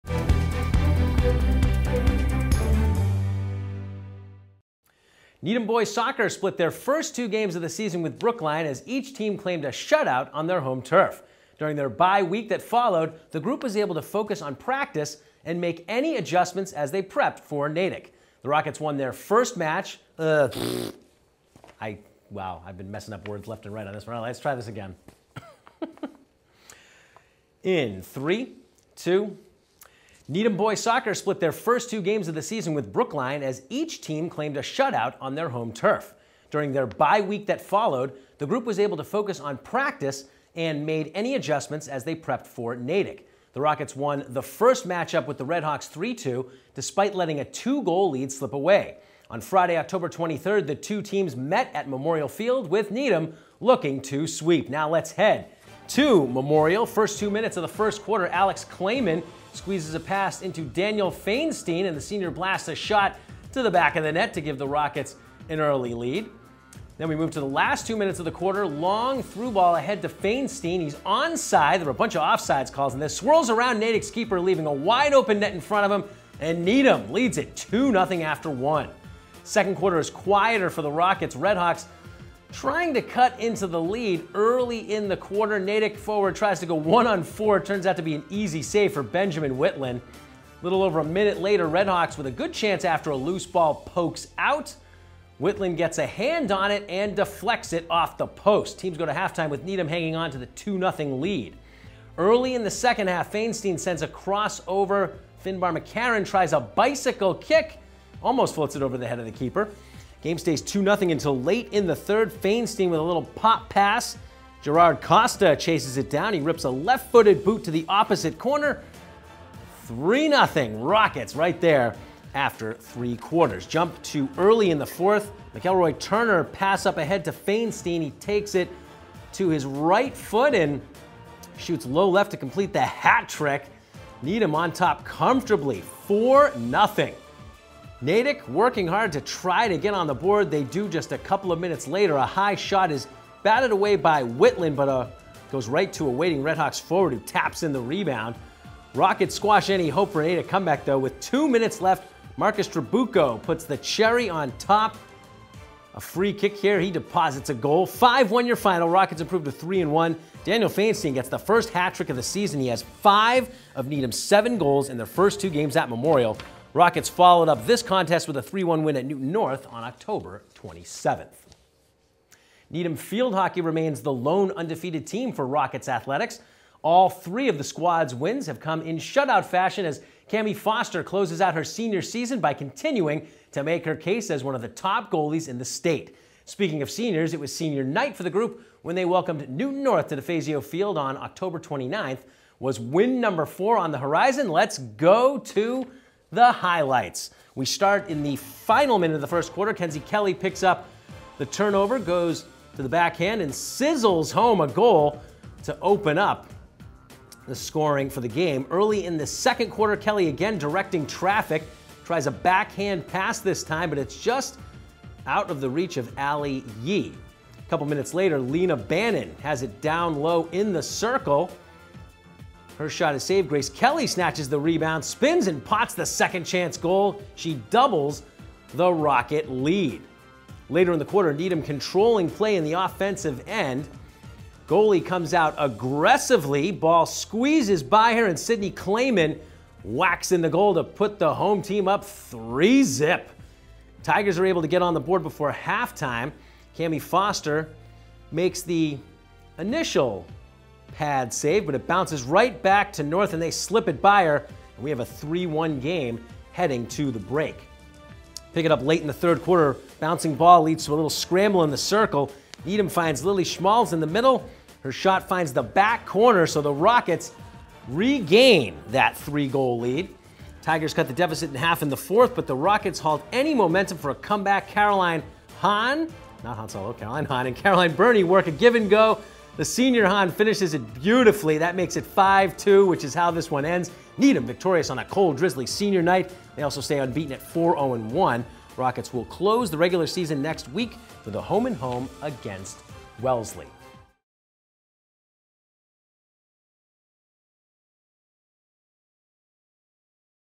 Sports Update - October 29, 2020
Highlights for Boys Soccer vs Natick and Field Hockey vs Newton North.